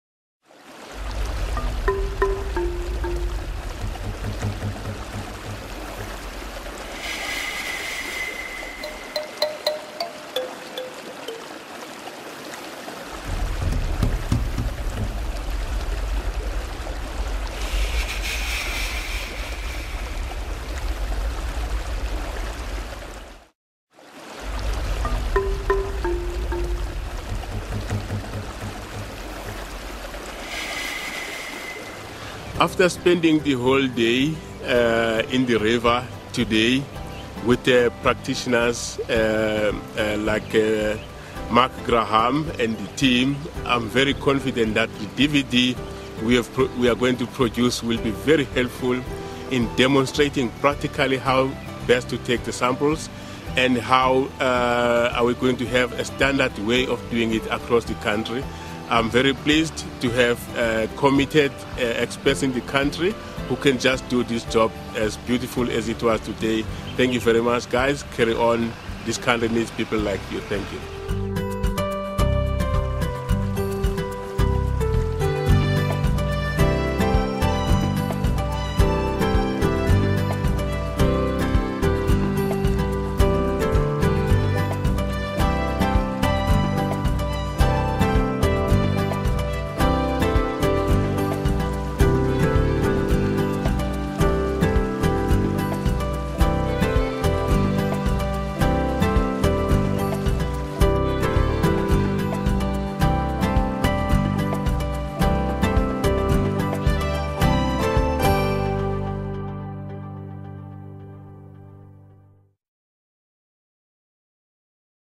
SASS5 Series - Additional information and conclusion
Some additional information on the video and organizations involved in SASS5 and closing address by WRC research manager Mr Bonani Madikizela